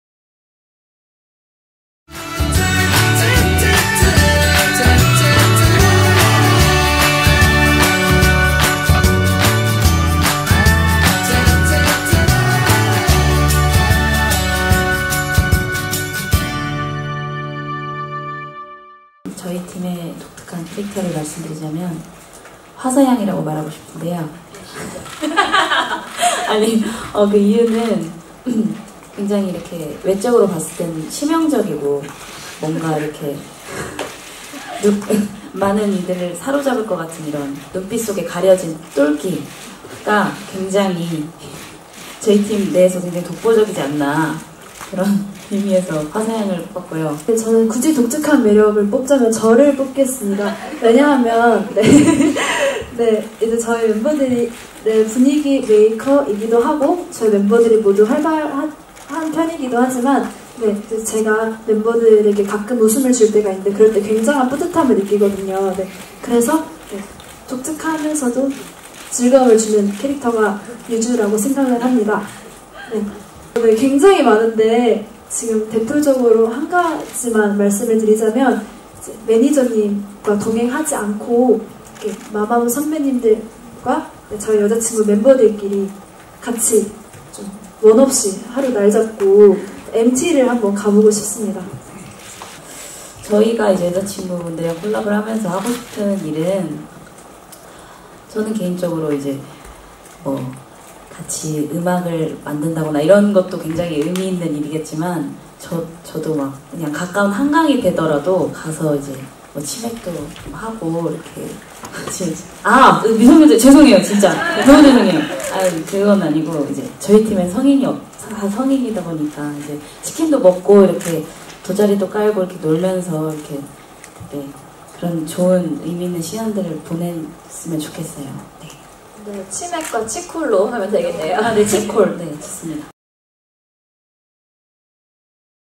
[TV TEN] 마마무&여자친구 SHOWTIME 제작발표회
MBCevery1 대표 예능 SHOWTIME 제작발표회 현장